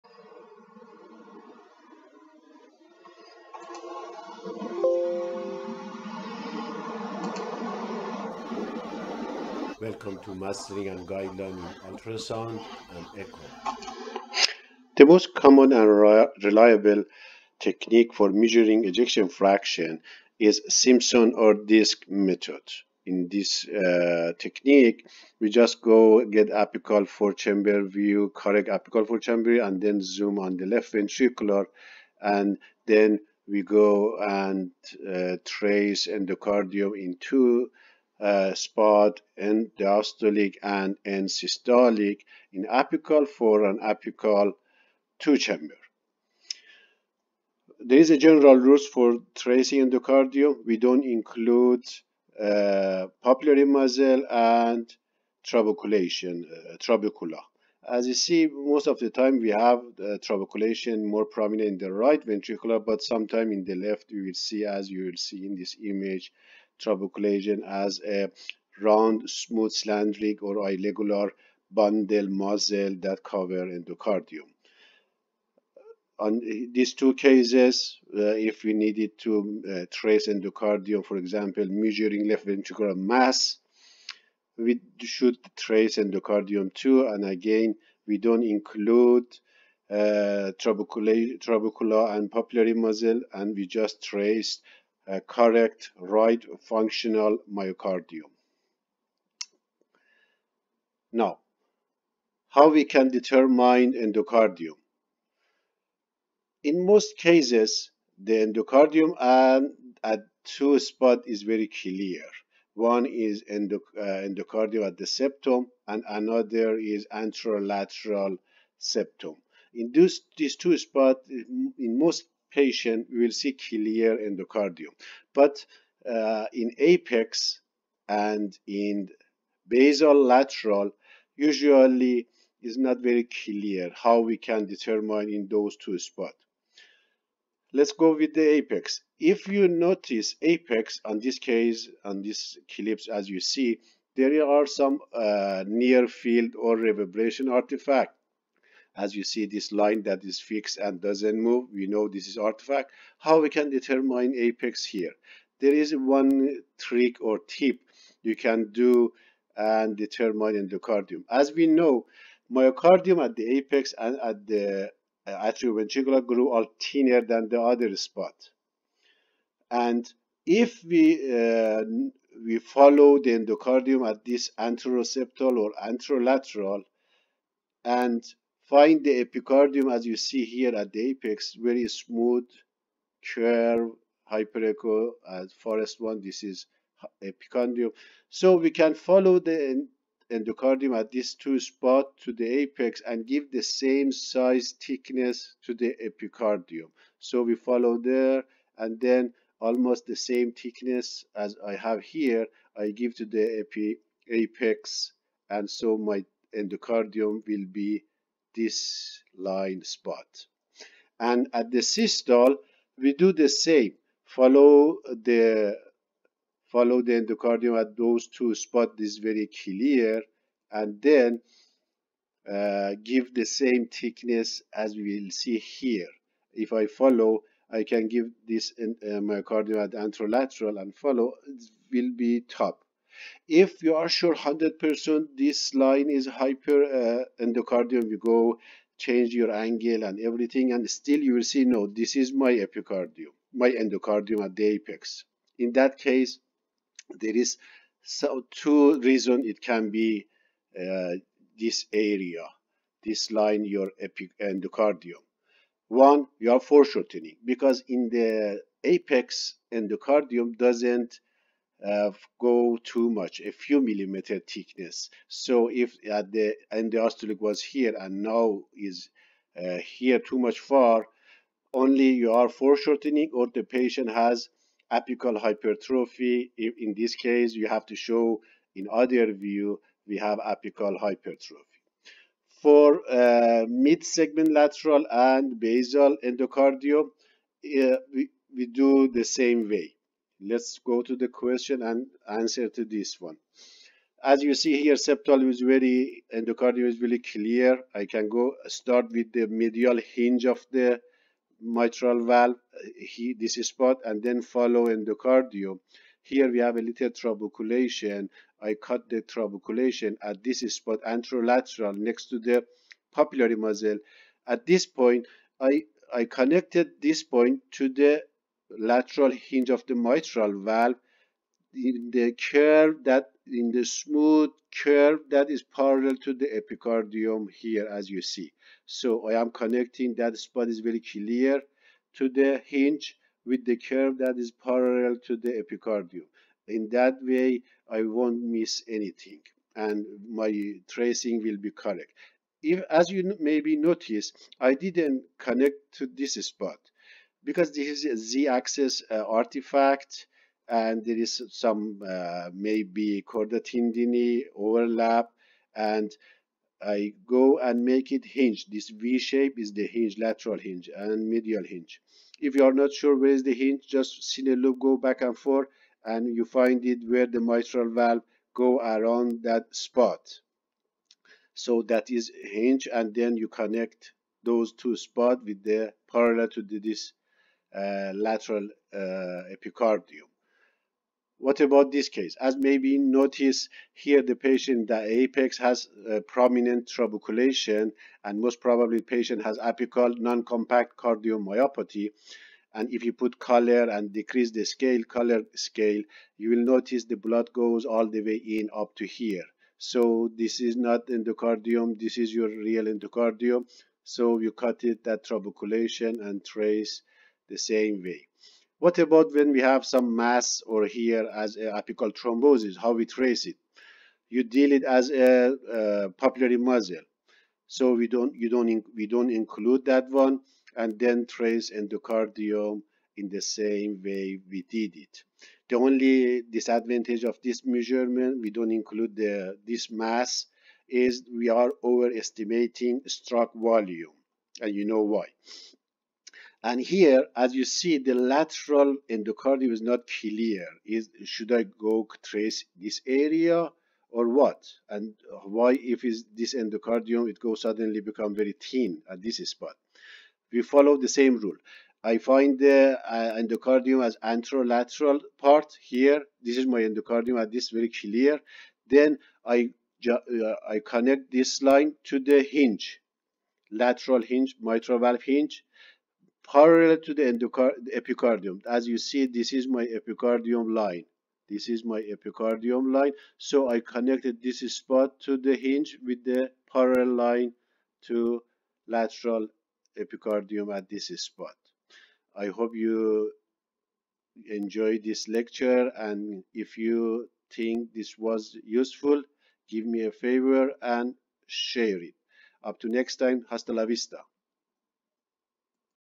answer to Q10 : pitfalls in tracing Endocardium
how we trace endocardium and its pitfalls
BY: Seyed A Sadatian MD. RDCS, RDMS. RVT